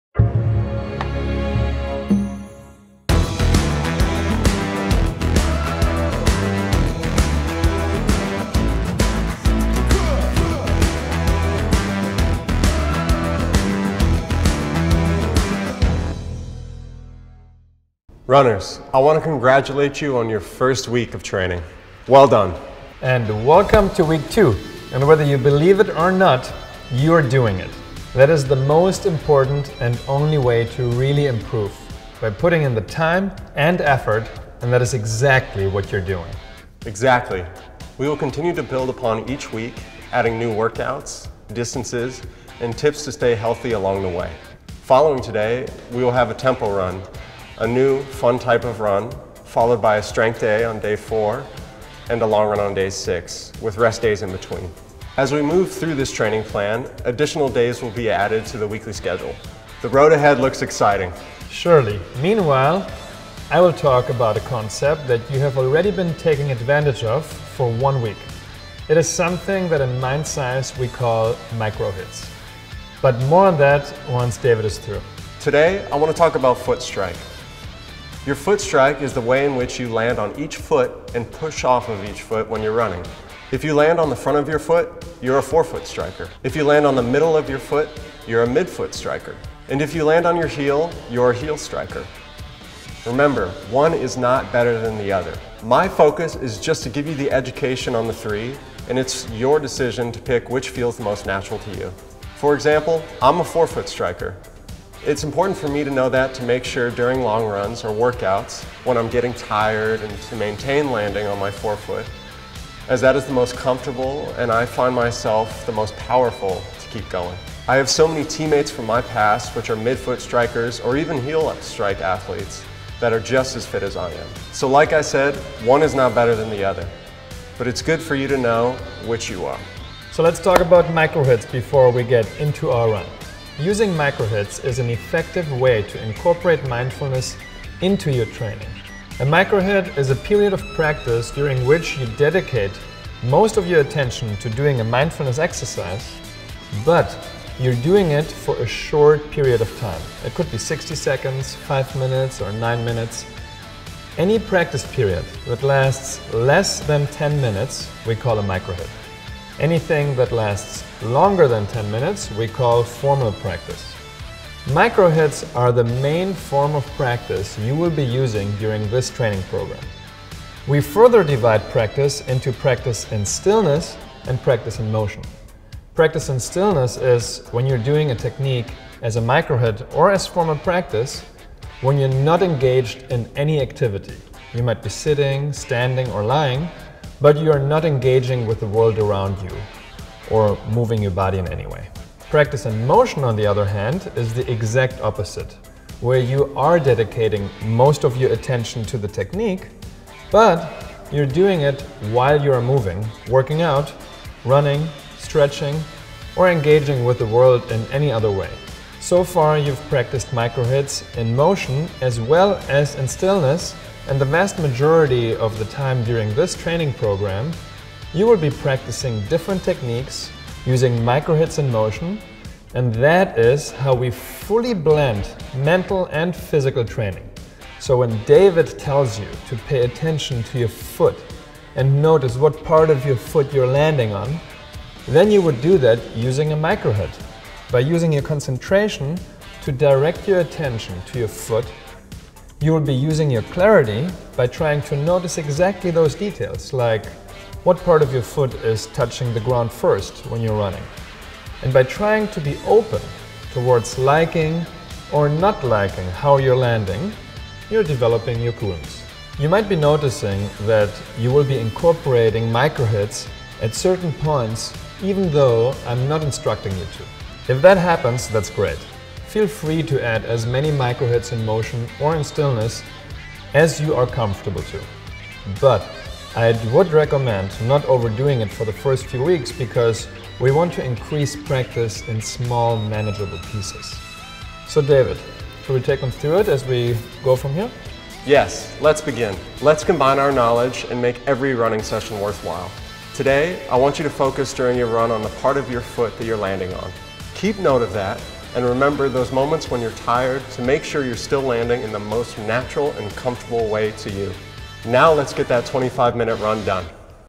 Revolutionise the way you run with base runs | Understand how base runs are key for marathons
Introducing the Mind & Body Marathon Training by Christian Straka and David Perry.

We are starting off with base runs and will be introducing tempo runs next to aid in achieving the 5k goal!

About the Instructors:
As a coach to pro athletes, Christian and David focus on mindset training for peak performance through evidence-based mindfulness skill development. They developed a unique approach to mindset training through methodologies for applying evidence-based mindfulness techniques in sports